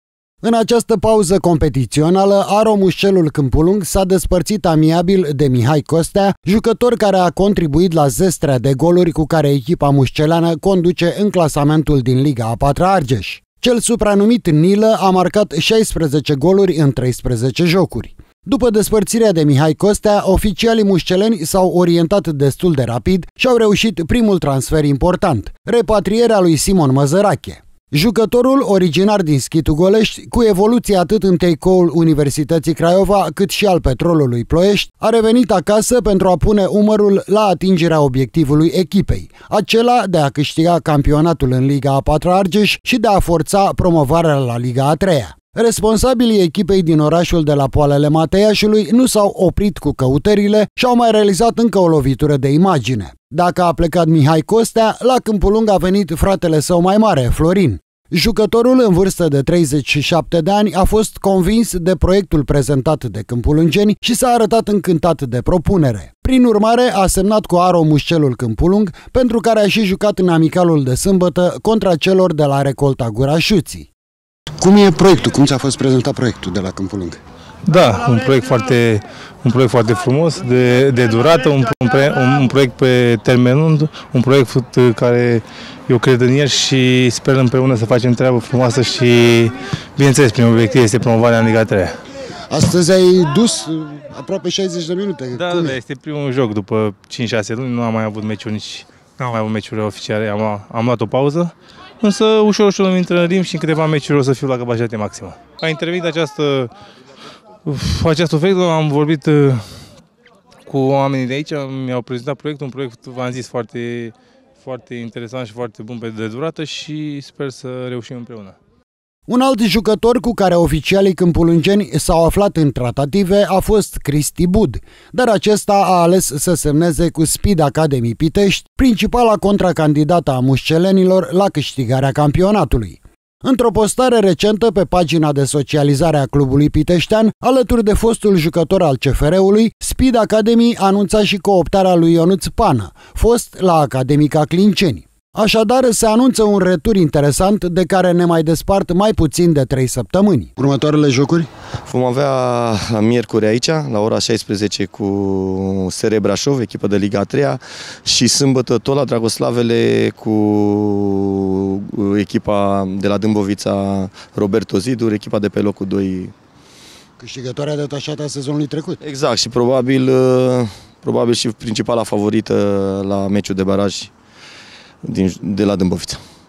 Întăriri pentru promovare ( 14 02 2023 )
Scurtă descriere:

Echipele din fruntea Ligii a IV-a Argeş se întăresc în vederea părţii a doua din acest sezon. Atât Aro Muscelul Câmpulung, cât şi Speed Academy Piteşti au primit întăriri în această pauză competiţională.